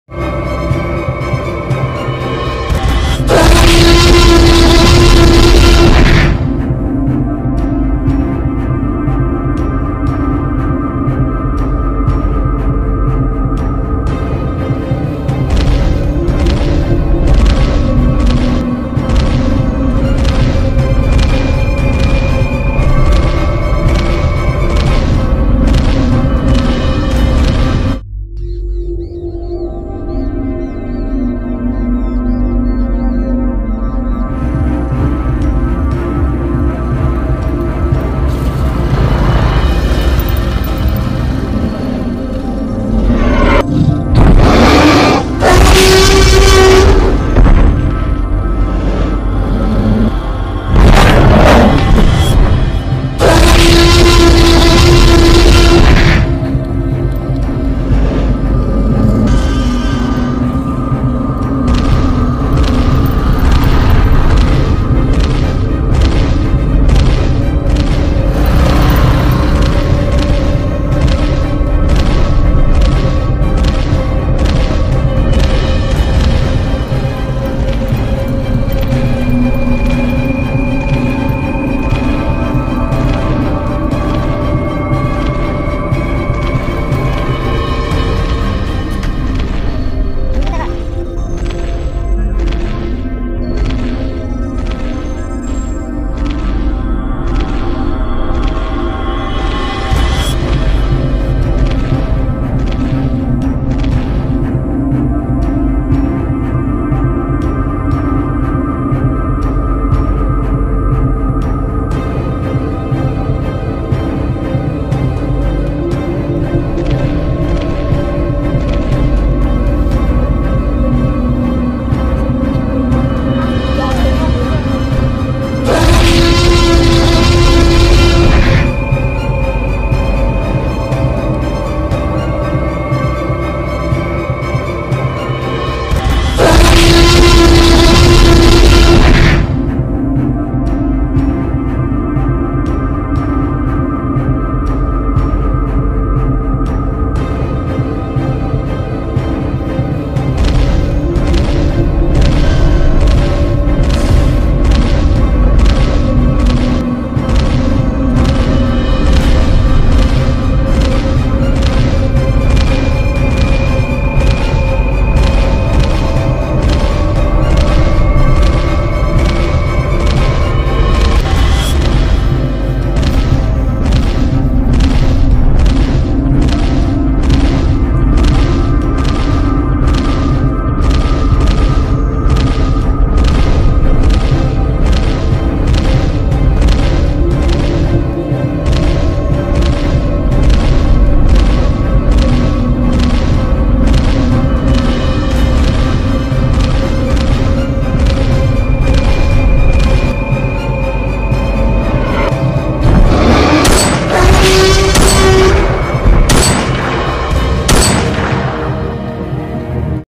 The BEST of Dinosaur Attack | Jurassic Park Fan Made Movie | T-rex Chase
The two brothers and sisters went for a ride on the scooter and saw the dinosaur then the dinosaur came behind them and left the scooter and hid behind them. Then they saw the dinosaur. On seeing the dinosaur, it attacked the boy and then followed the girl.
A man came to save him by shooting the dinosaur but the man Gorjira came and destroyed the dinosaur.